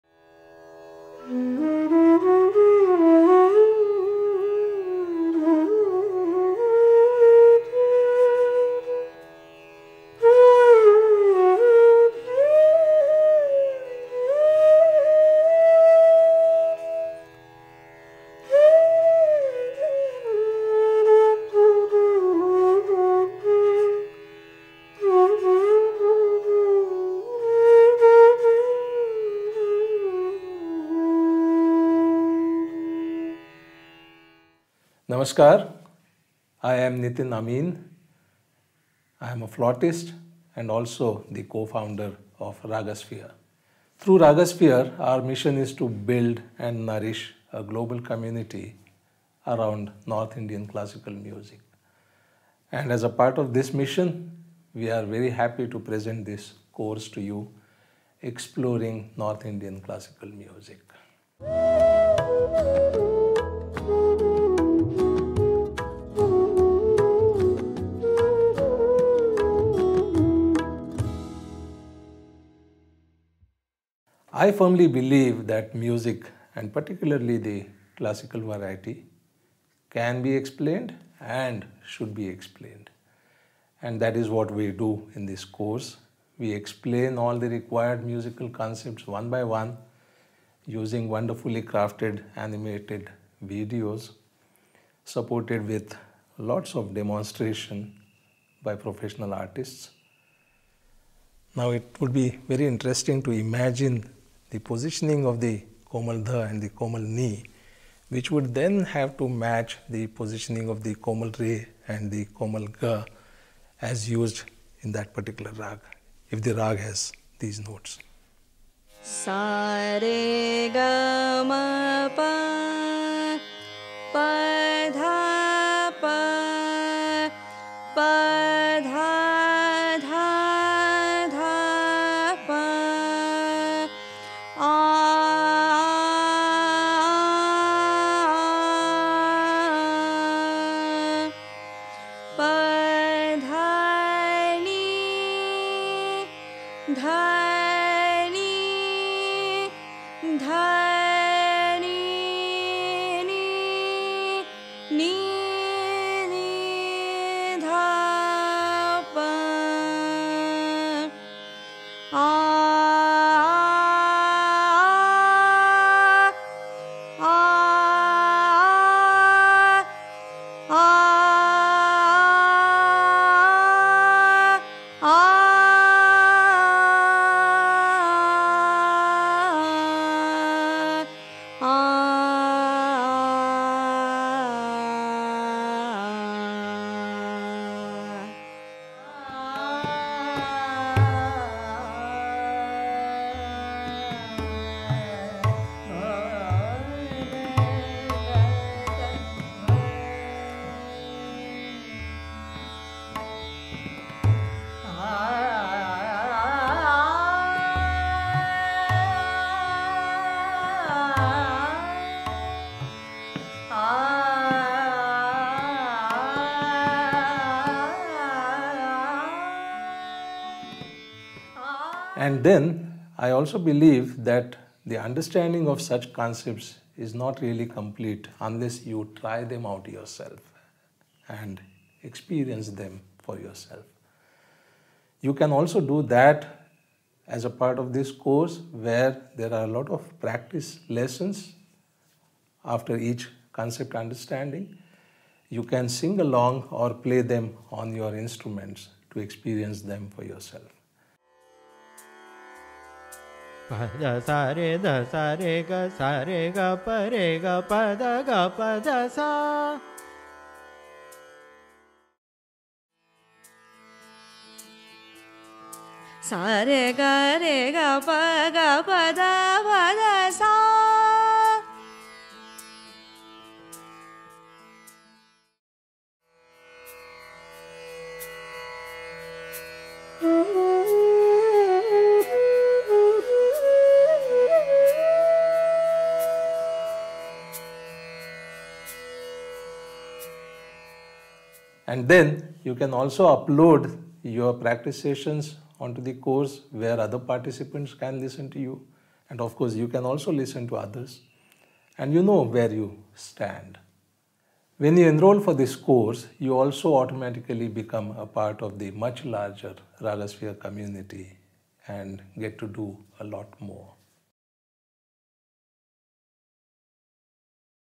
Exploring North Indian Classical Music
This Sunday, I thought of sharing a bit more about our course for understanding and practicing North Indian Classical Msuic...
Will of course resume our Raga Parichay next week.